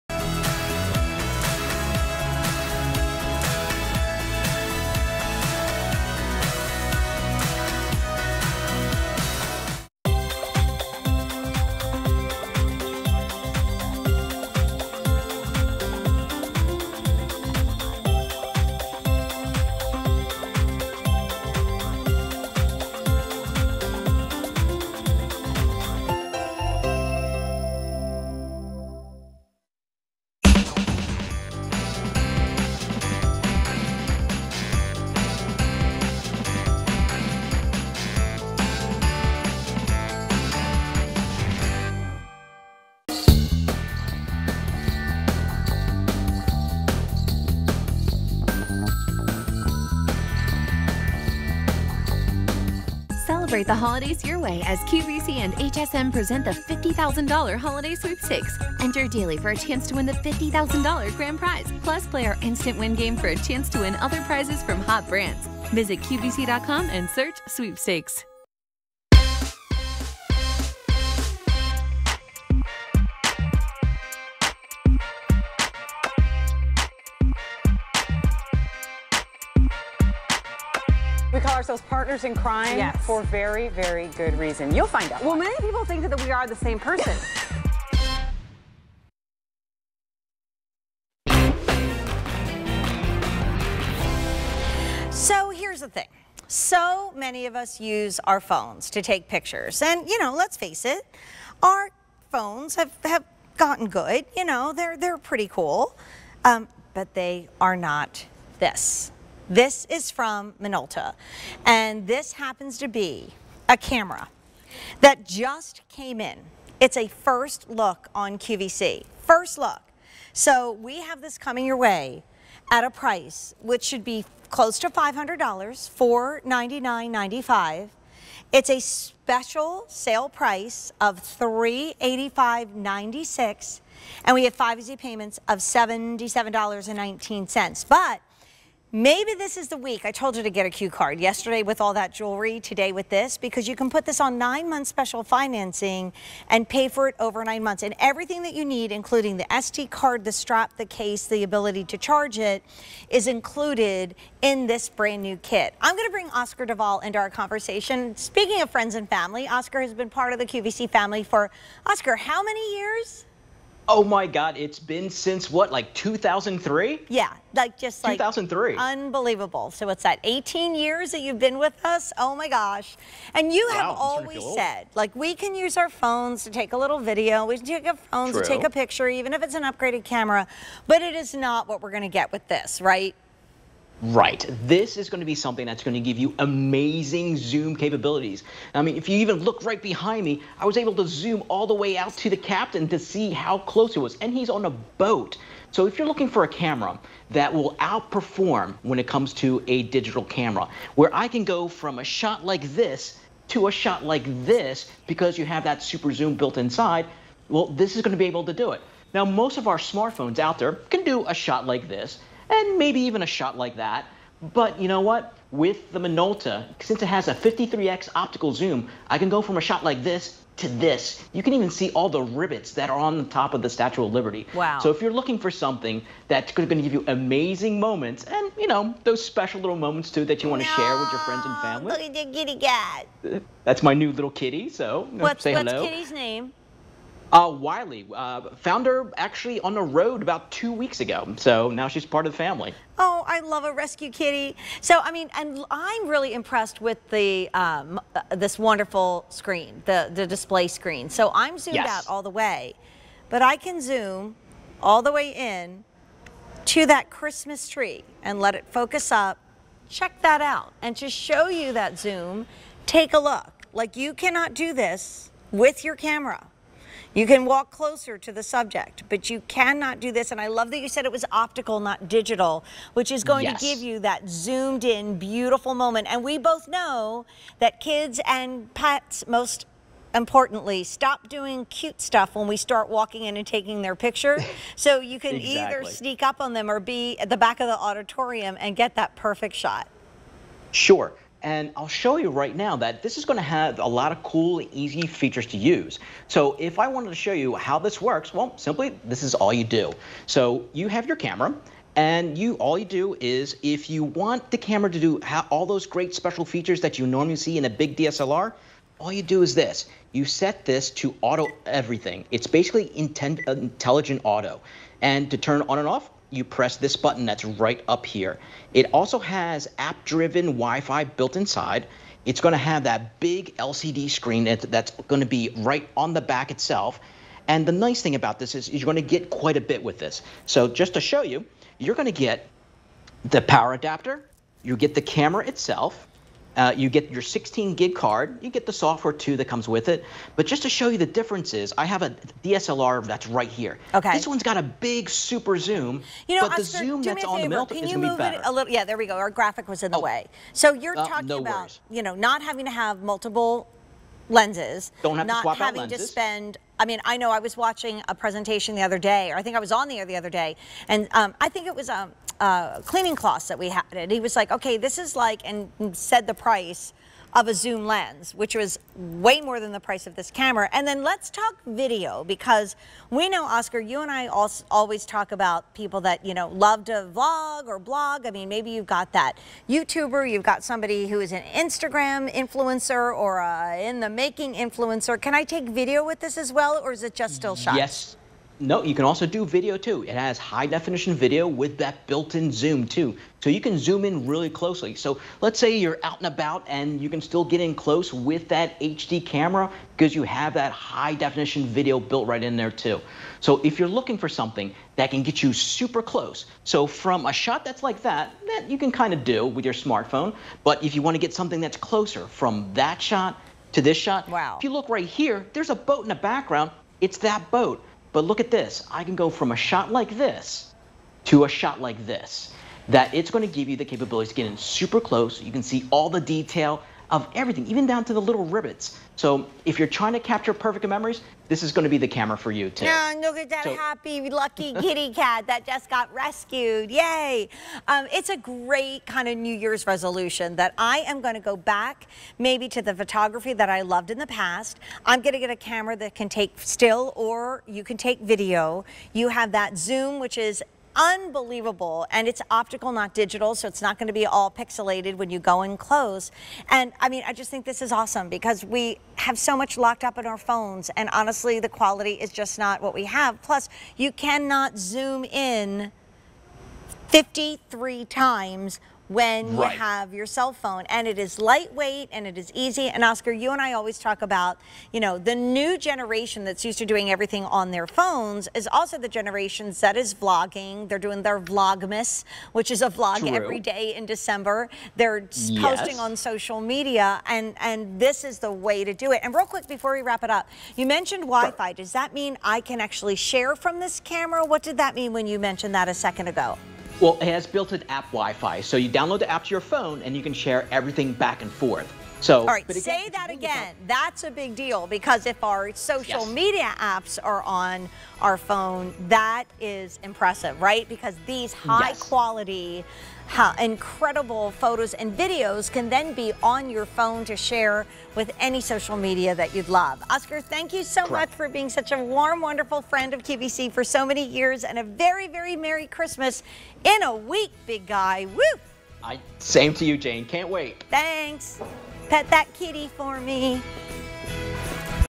Minolta MN53Z Optical Zoom SLR Camera with 16GB SD Card on QVC
Minolta MN53Z Optical Zoom SLR Camera with 16GB SD Card

Don't trust those memorable moments to anything less than this Minolta MN53Z digital camera. Capture impressive 20MP stills, record Full HD video for rich detail and vibrant color, and easily share your photos with other Wi-Fi-enabled devices. The 53X optical zoom makes shooting from afar or capturing an intimate close-up a breeze. Plus, user-friendly features like face, smile, and blink detection make it easy to snap the perfect shot. From Minolta.